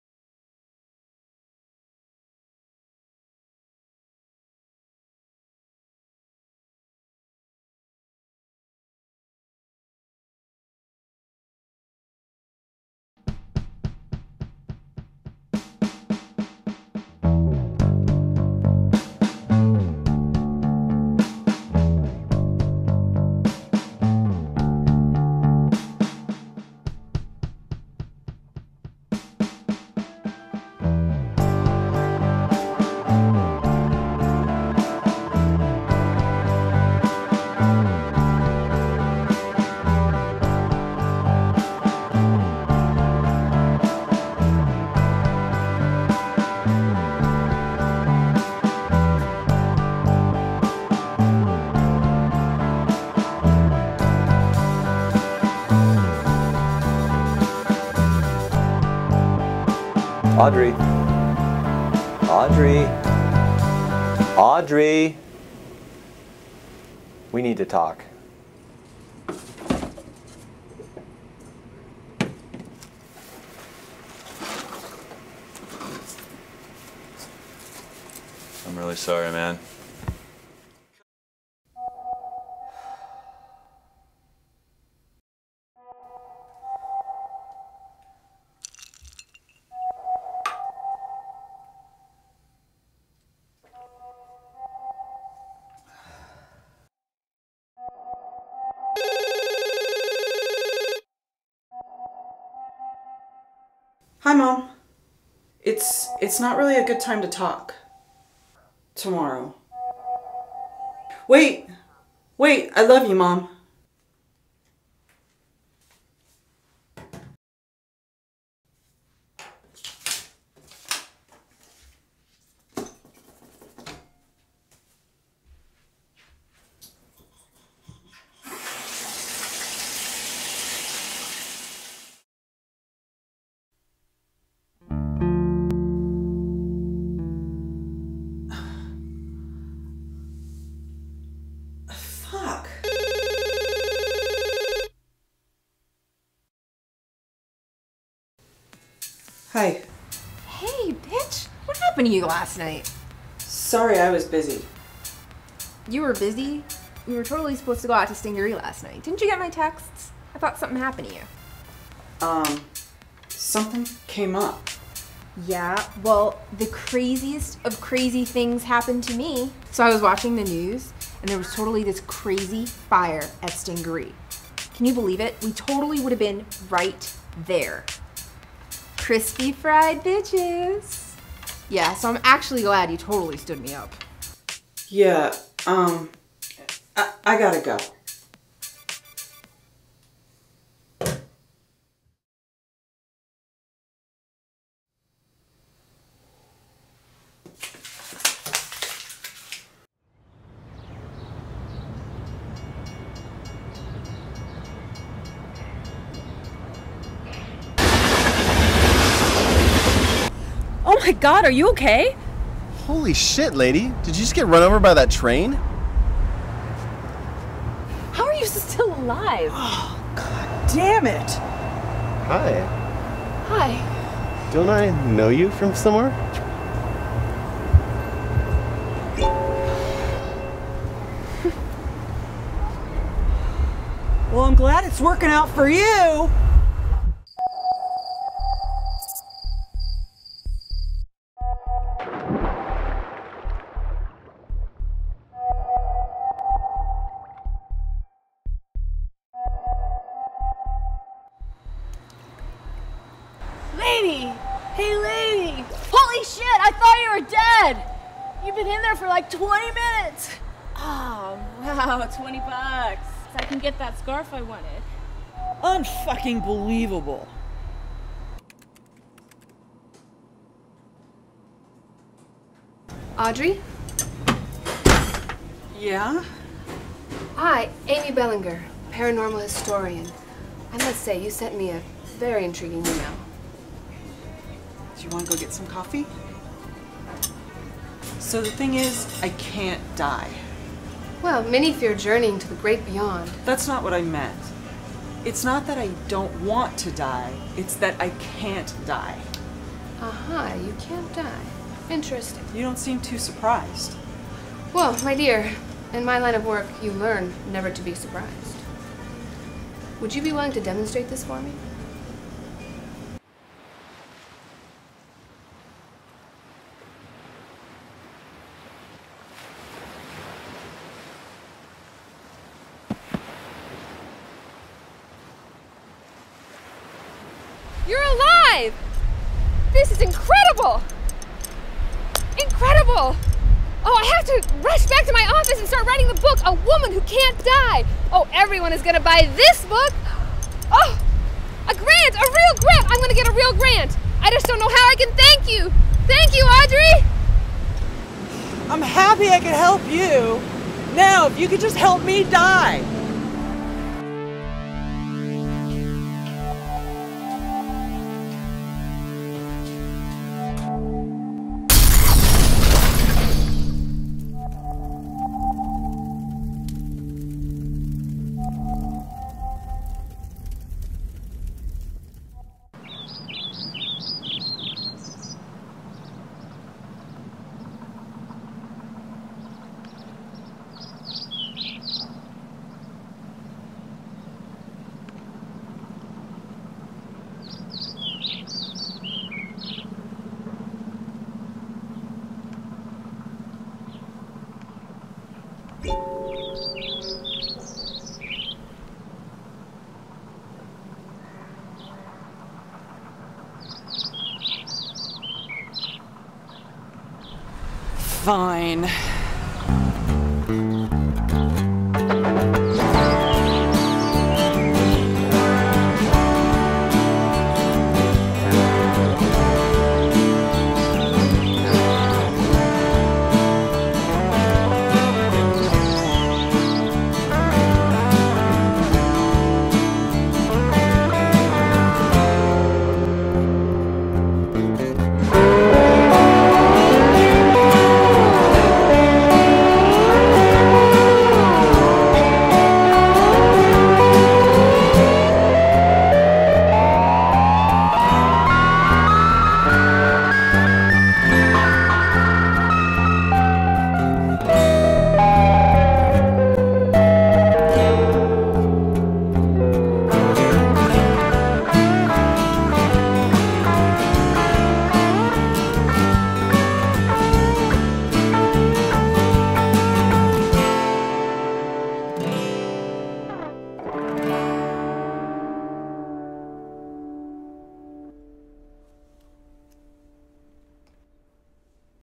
Daisy Chain
by That's Rad Productions

Our entry into the 48 Hour Film Project 2009 San Diego.

Genre: Drama
Character: Amy Belanger
Prop: Crayon
Line of Dialog: "That's not what I meant"